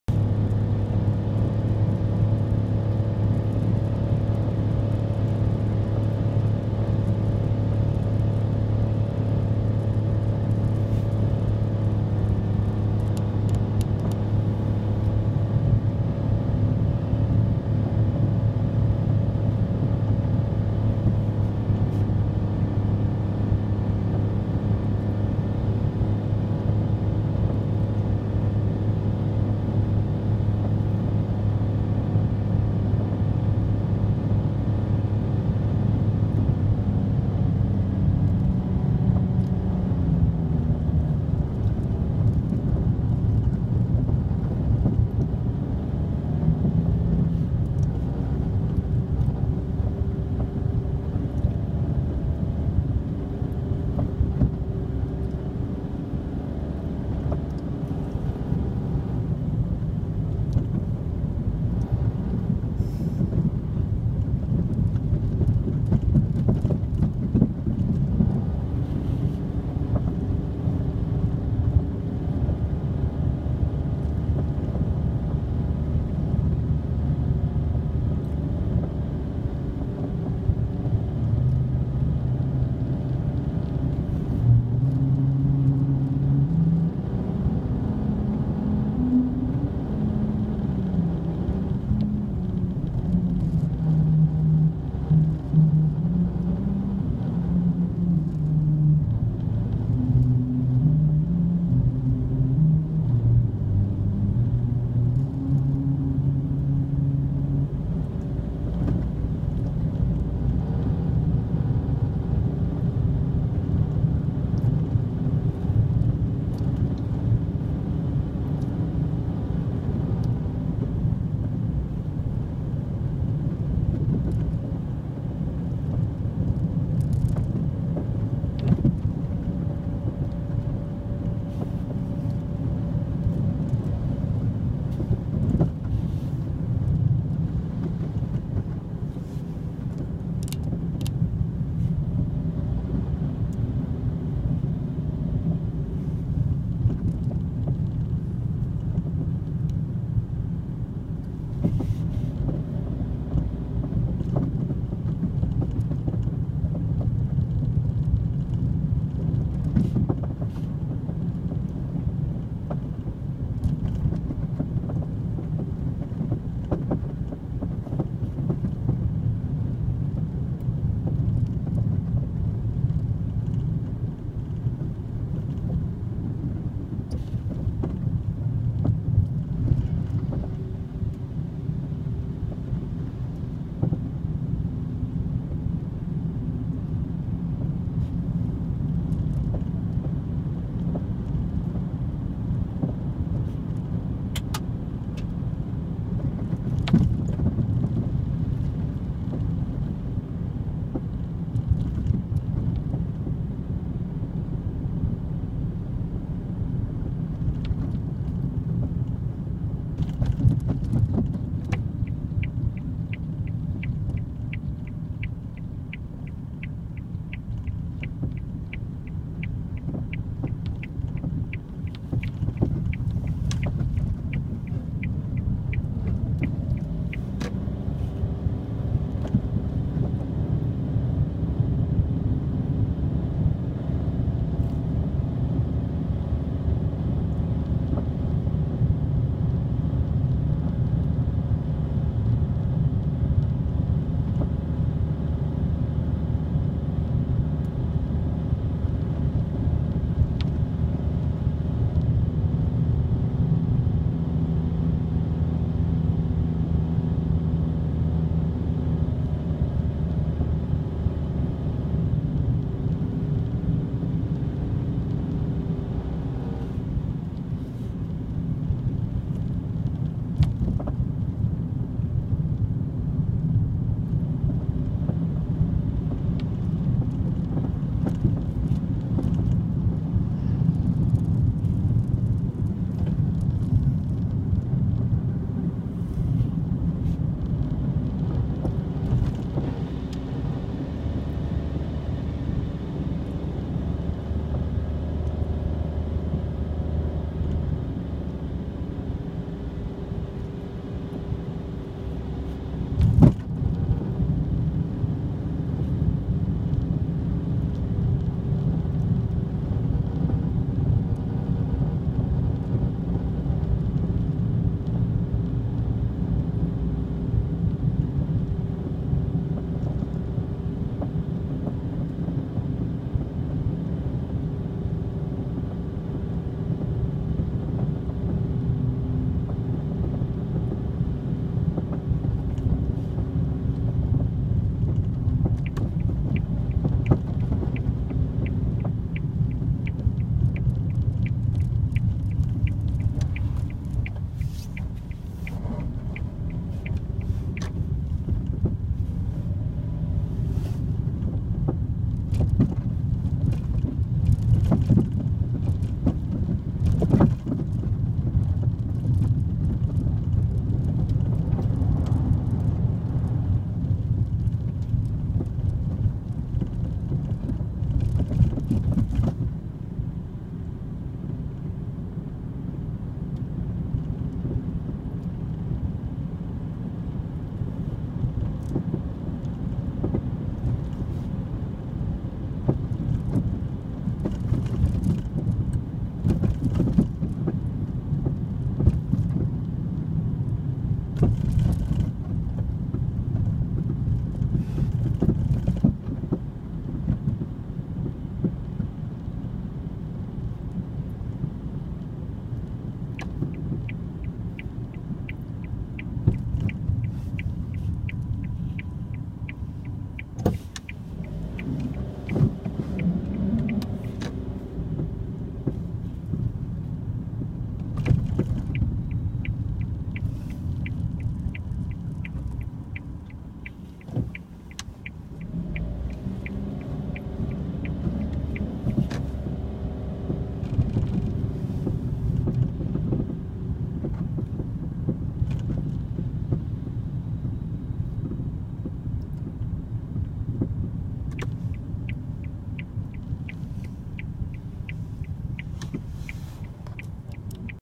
草津温泉 道路状況 2018.2.11 6時半頃 晴れ -1℃ 国道292号線
今朝は晴れです。昨夜は、思ったほど雪は降りませんでした。しかし道路は凍結しております。お気を付けてお越しくださいませ。